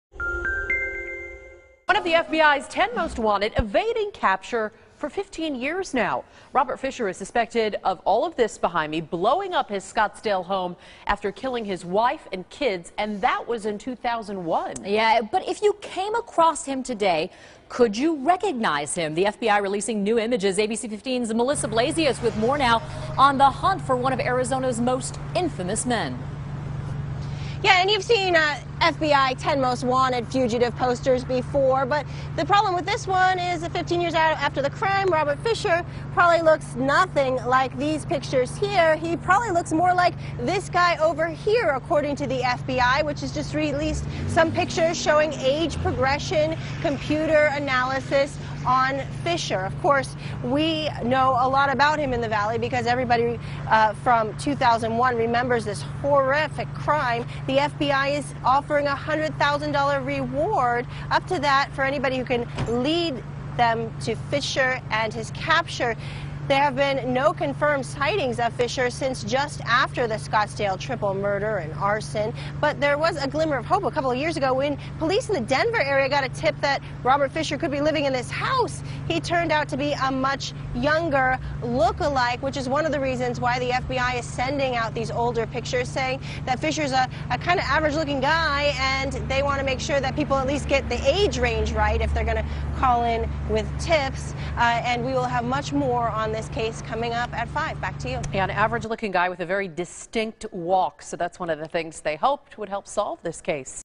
The hunt for Robert Fisher
FBI releases new photos of wanted man.